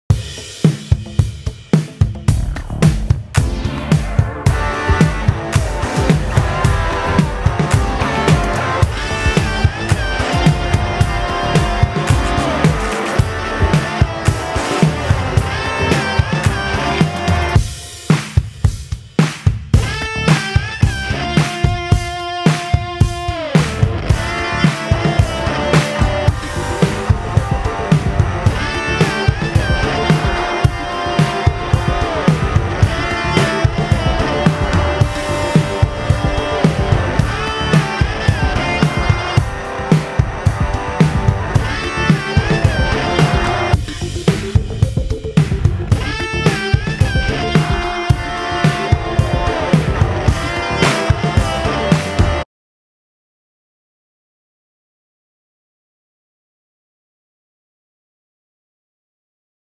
Riff
We have been working on mixing some toons /audio , so we put them with some short videos we got  from Pixelbay.com so we can get some public feedback .